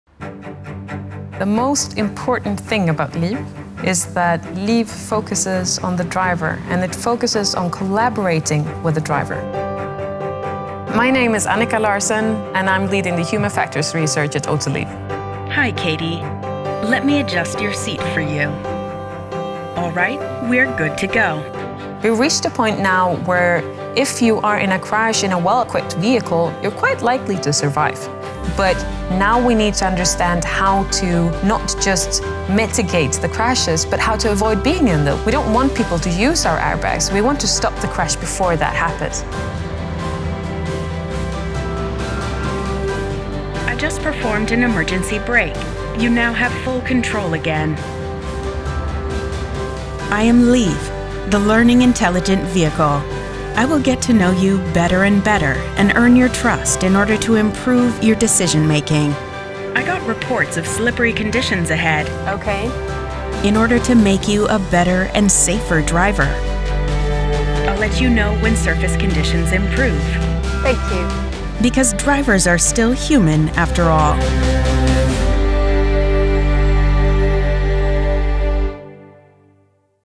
Launching LIV, the Learning Intelligent Vehicle | Autoliv
LIV, the Learning Intelligent Vehicle, is developed by Autoliv Research to facilitate collaboration and shared control between driver and vehicle. It focuses on the driver, and collaborating with the driver, to improve decision making and stop the crash before it happens. This in order to attain trust and facilitate required safety in autonomous driving.